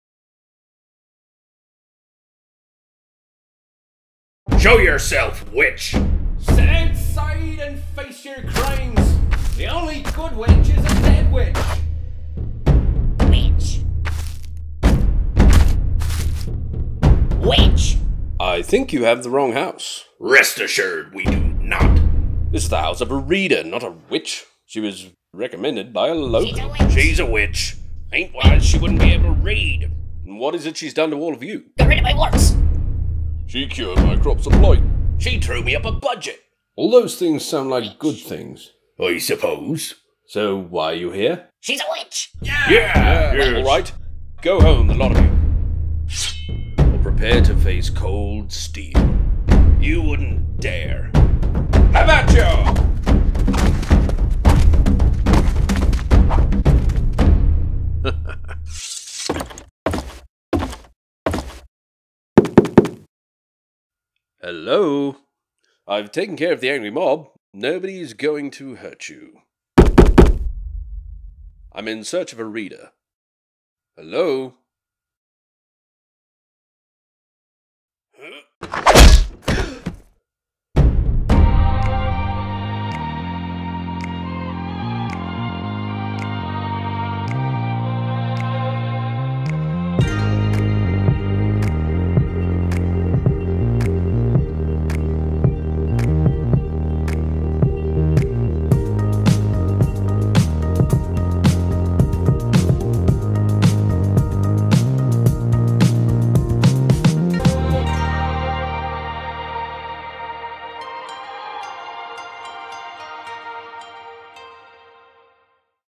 Inquisition - RodeReel2018
A knight stumbles upon an angry mob on his journey to find a Reader, he must calm the situation before anybody gets hurt.